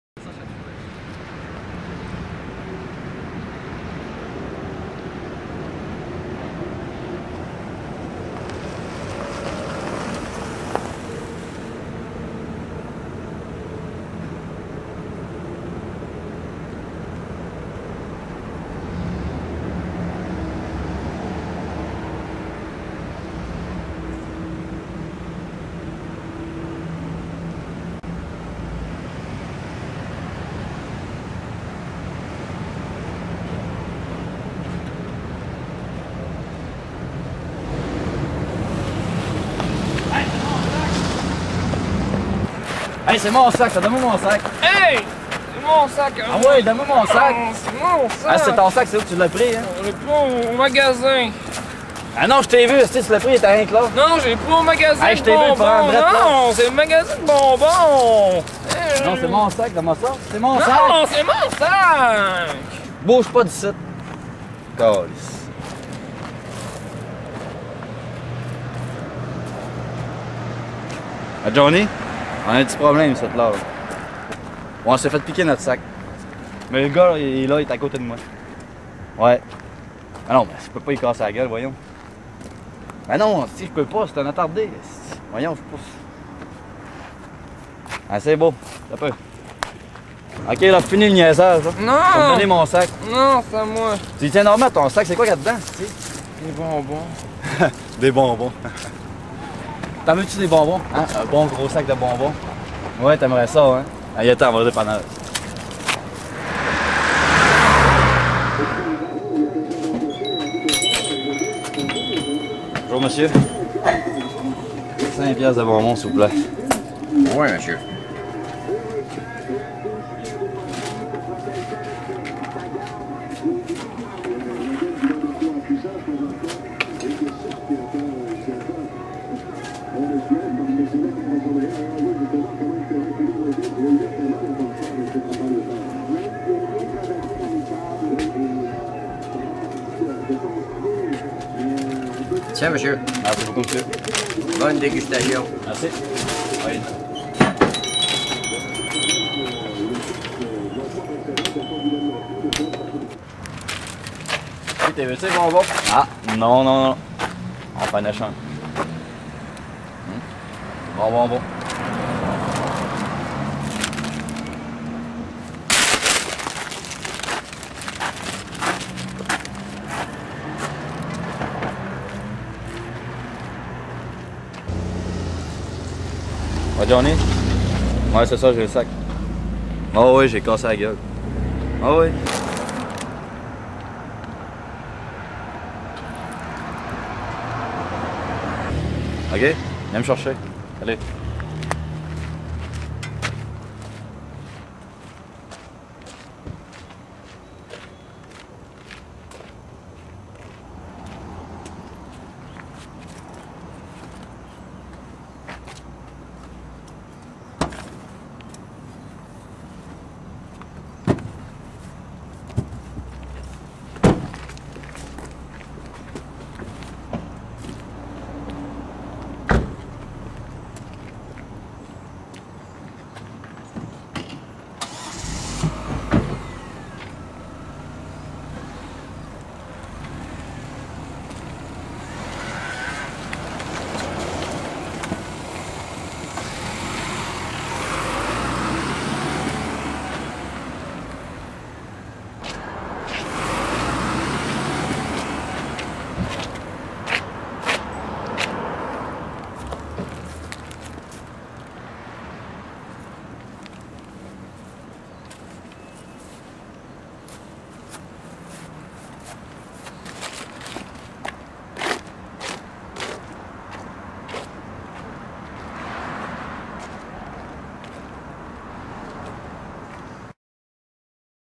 Sac à surprises
3reg de L'Organisation Laborieuse du Film
Thème: Sac à dos
Réal: François Morin
Avec: Gabriel Tremblay et J.J.
© 3REG - O.L.F.